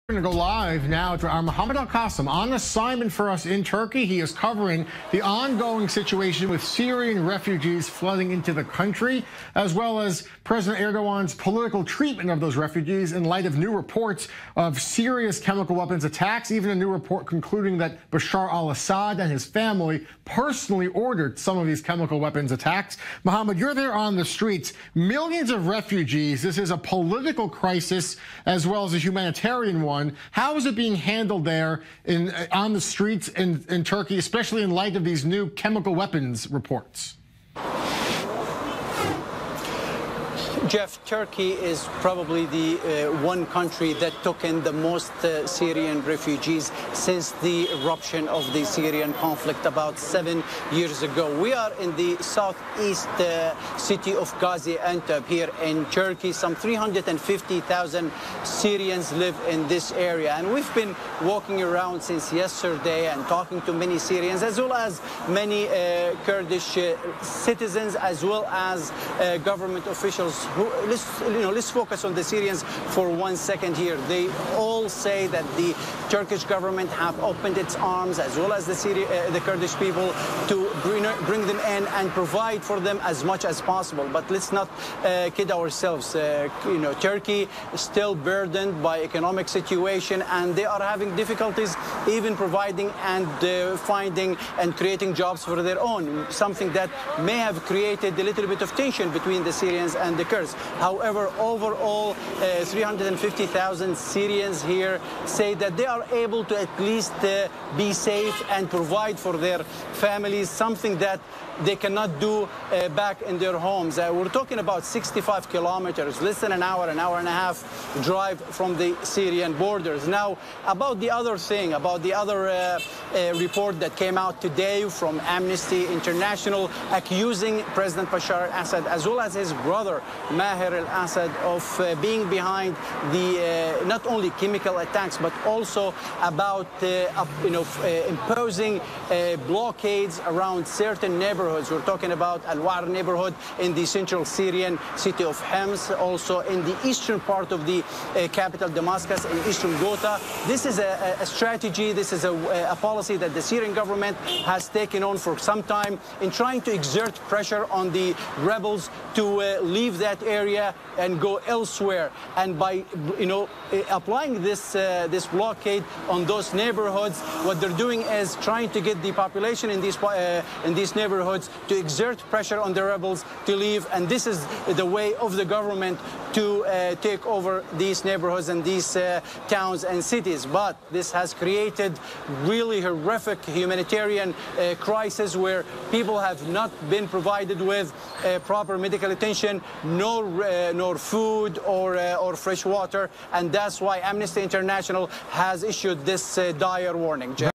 Our Mohammad Al-Kassim is live in Turkey, speaking to some of the over 3mill Syrian refugees
Our Mohammad Al-Kassim is live in Turkey, speaking to some of the over 3 million Syrian refugees in the country.